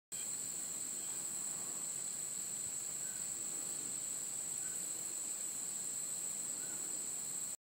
White-lined Sphinx Moth
Feeding on thistle flower on an Iowa tall grass prairie.